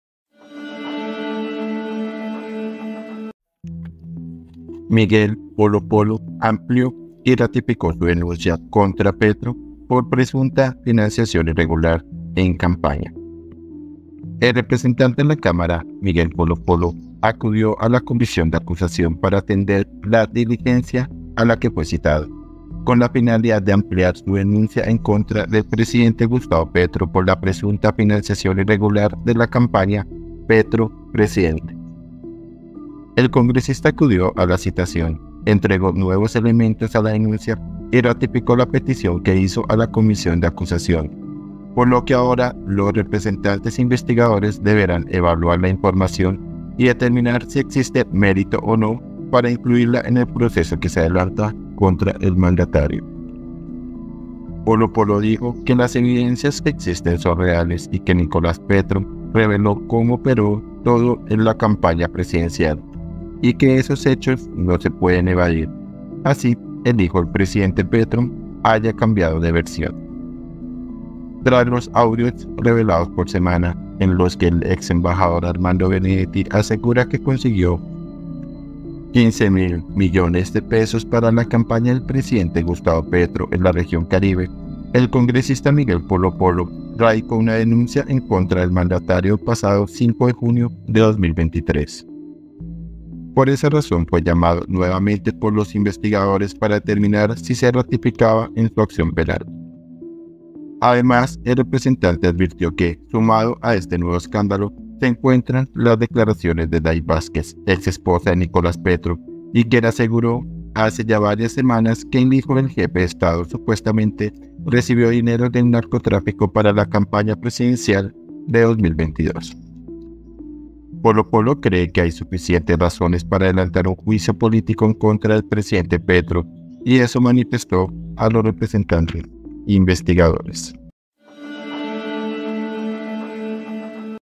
🛑 POLO POLO AMPLIÓ Y RATIFICIÓ SUS DENUNCIAS CONTRA PETRO
El representante a la Cámara Miguel Polo Polo acudió a la Comisión de Acusación para atender la diligencia a la que fue citado, con la finalidad de ampliar su denuncia en contra del presidente Gustavo Petro por la presunta financiación irregular de la campaña Petro Presidente.
CONFIDENCIALES SEMANA